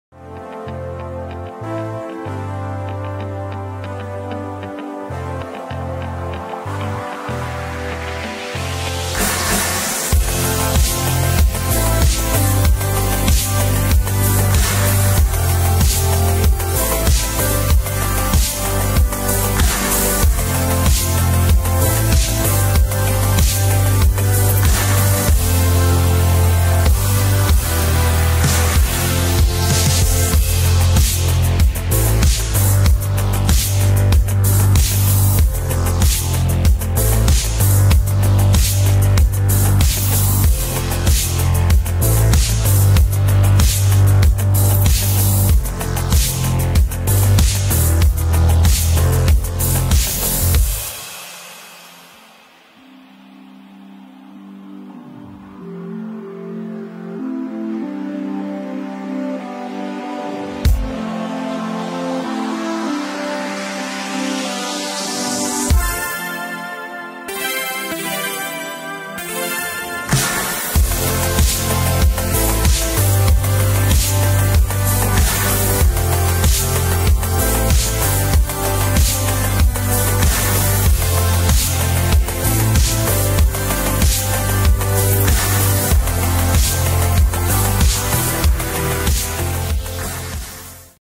Berlin Impressions Tempelhof Airport DC-3 Aircraft Airlift Anniversary
Tempelhof Airport THF Berlin Impressions